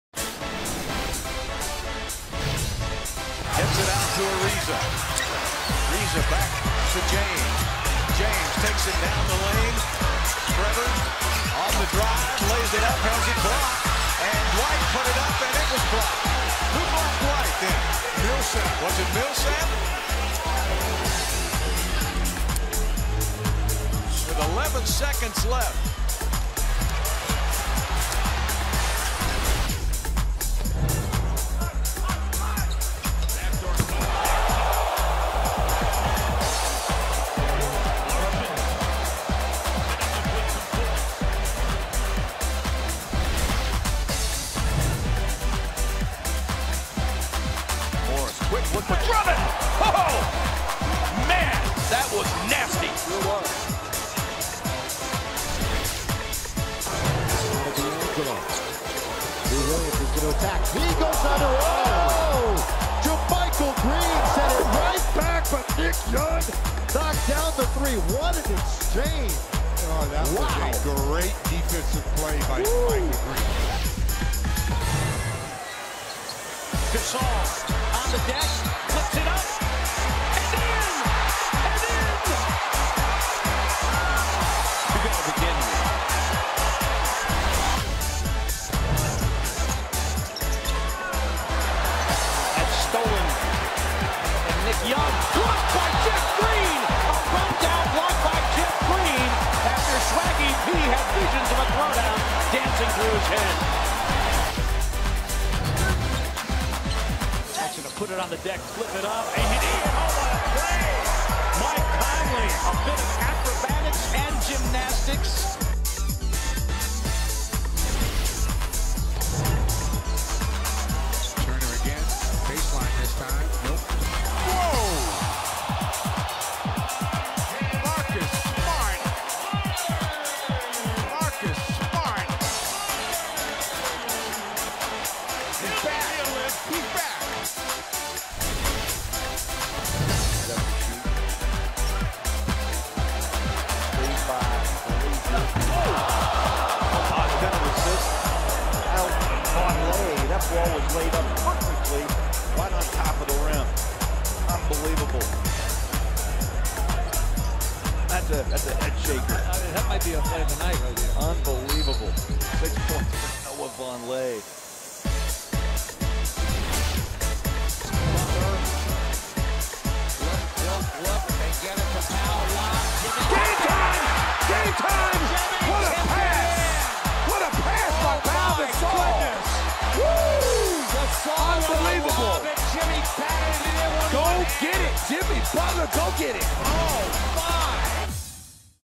NBA Top 10 Plays of the Week | January 3, 2016 | NBA 2015-16 Season
NBA.com presents the top 10 plays of the week.